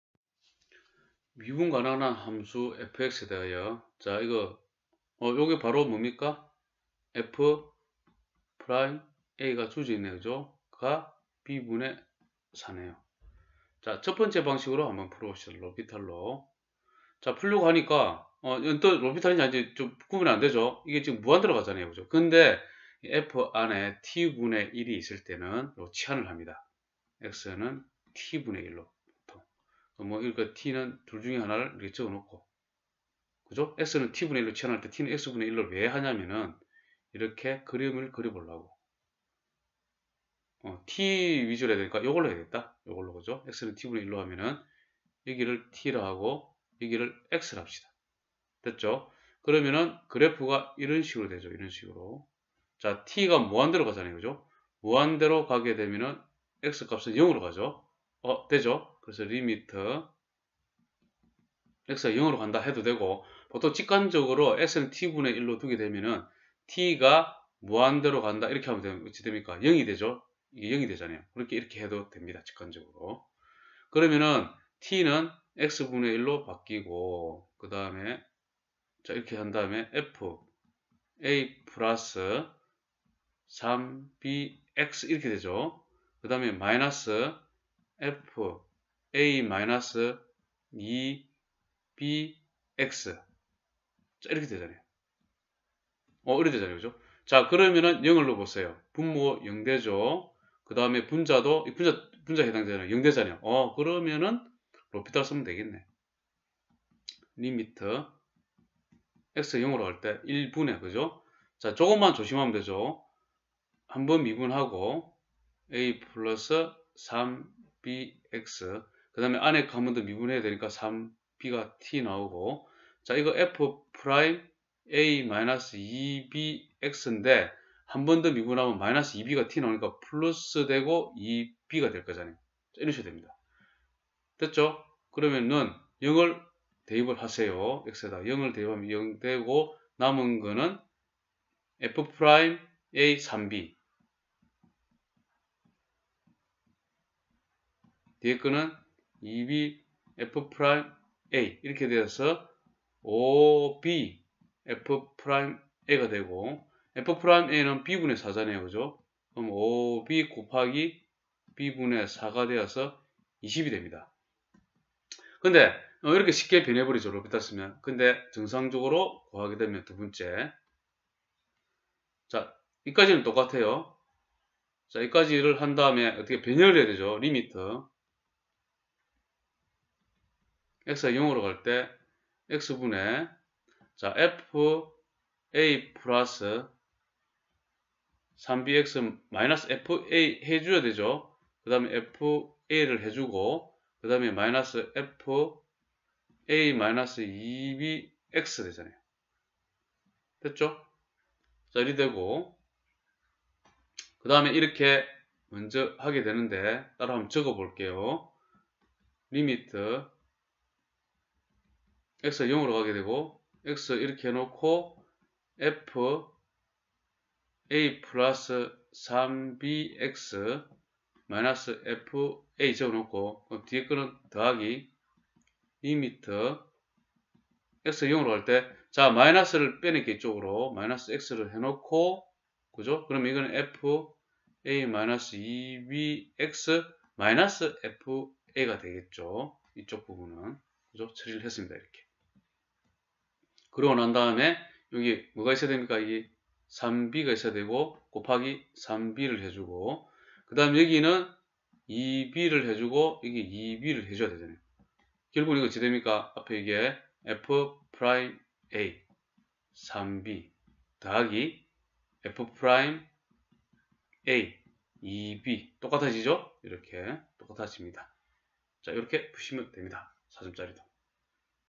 [백년지계]MailStudy 고등수학 미분법 문제 6 Level 4(MailStudy Math derivative 6 Level 4)
1. 로피탈로 Level4 문제를 Level2 처럼 6탄
2. 한 문제씩 풀어보는 문제
4. 추후 Level 선택에 따른 문제 제공